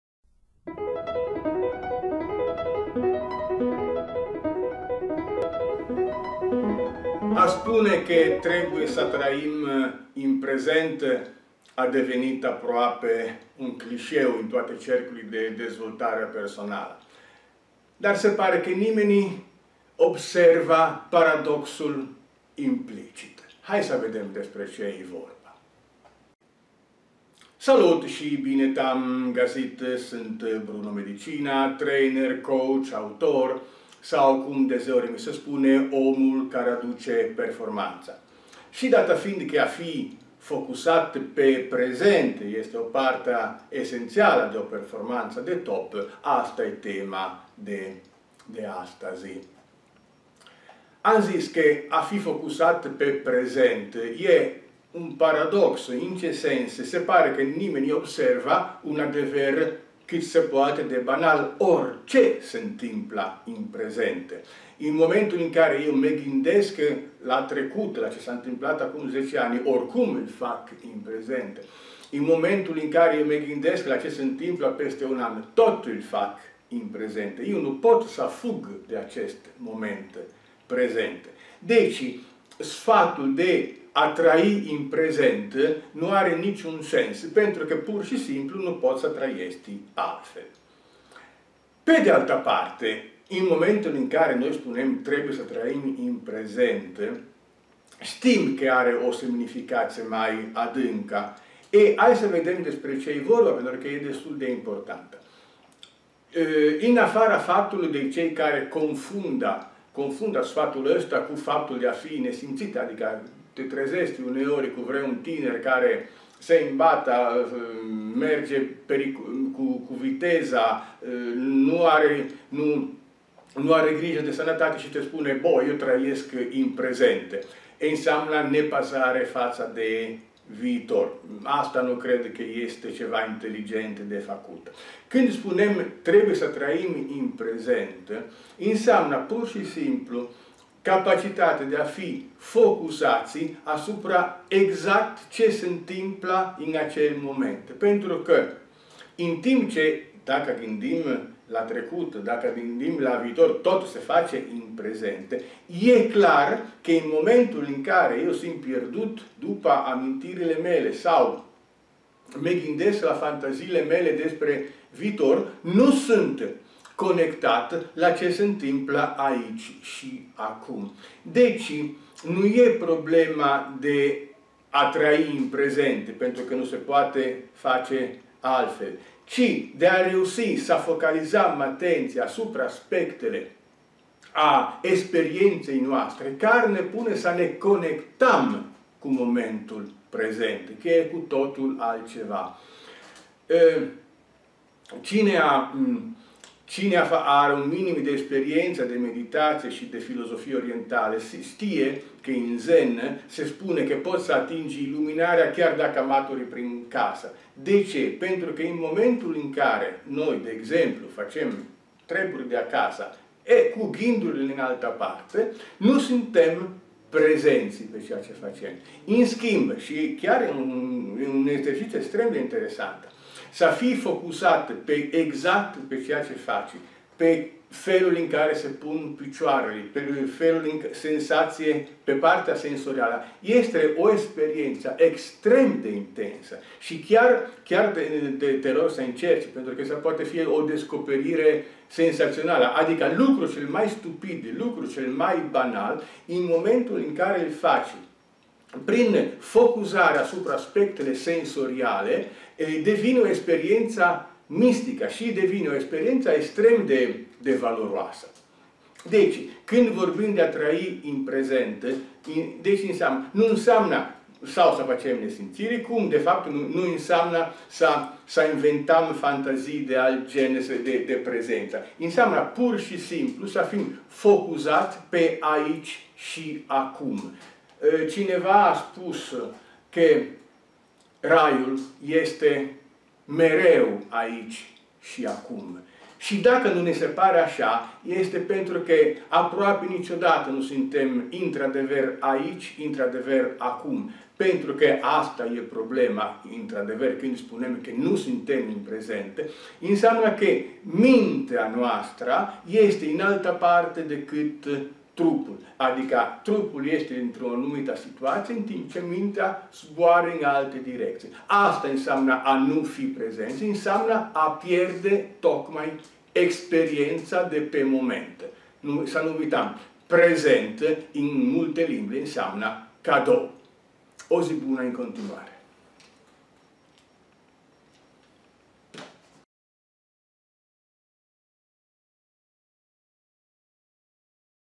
Mitul „trăirii în prezent”
A spune că trebuie să trăim în prezent a devenit aproape un clișeu, dar nimeni nu pare să observe paradoxul implicit.